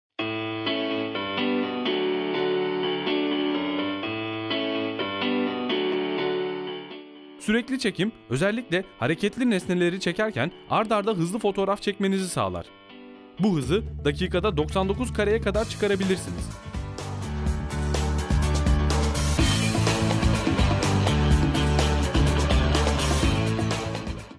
45 - General Mobile Discovery-Hızlı Fotoğraf Çekim Özelliği
Sürekli çekim özellikle hareketli nesneleri çekerken ard arda hızlı fotoğraf çekmenizi sağlar.
Dakikada 99 kareye kadar arttırabilirsiniz.